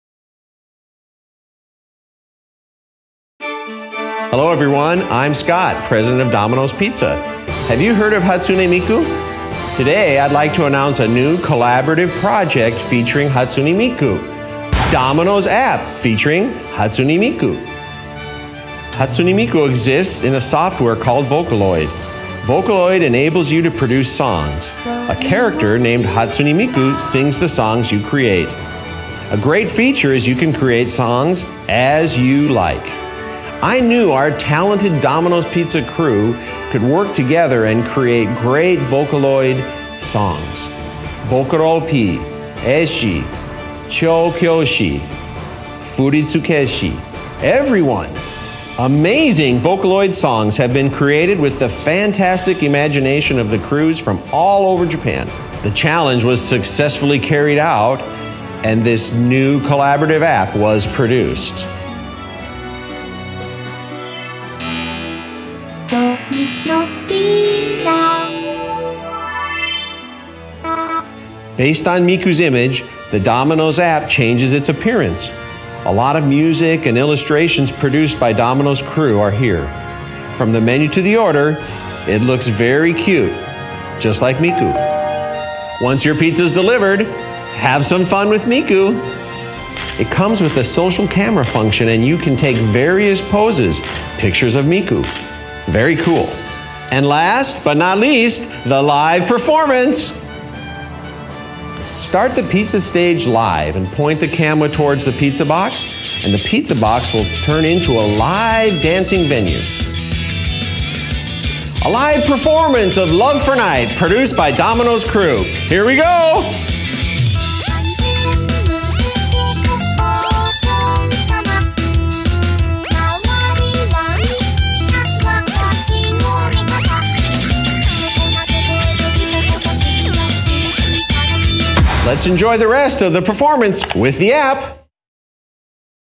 Domino's App Feat Hatsune Miku (Original)
Domino's app featuring Hatsune Miku.

(Has Been Since Taken Down)

Edit 2:
This was real in Japan. Scott retired and this got removed.